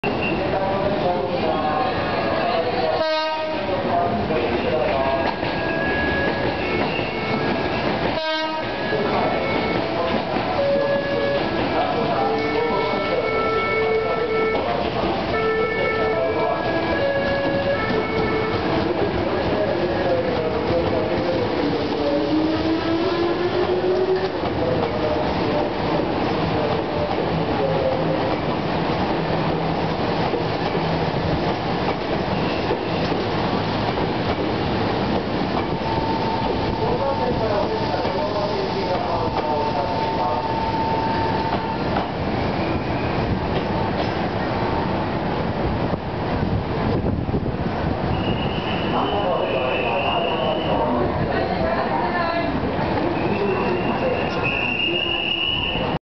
名鉄 7000系 パノラマカー ありがとうP6の回送 金山駅 待避線へ
撮影日2008/09/14
イベントを終えて進行方向を変えるために金山駅の待避線に向かうところです